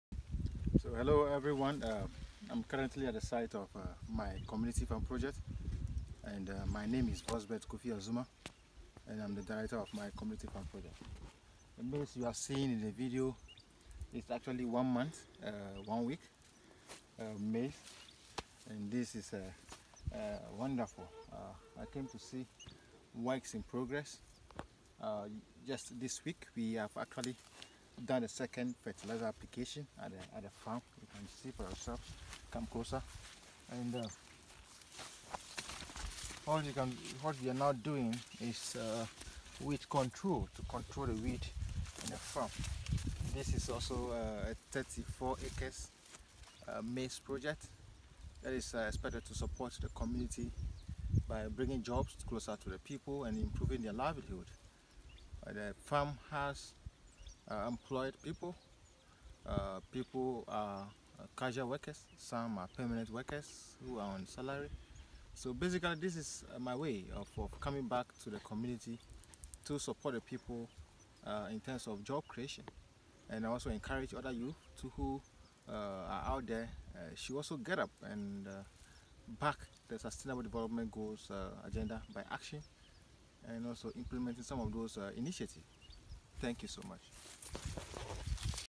Vozbeth Kofi Azumah visit his 35 acres maize project
After 1 month 1 week of planting our hybrid maize at My Community Farm Project- Mycfp. The result is here for you to see.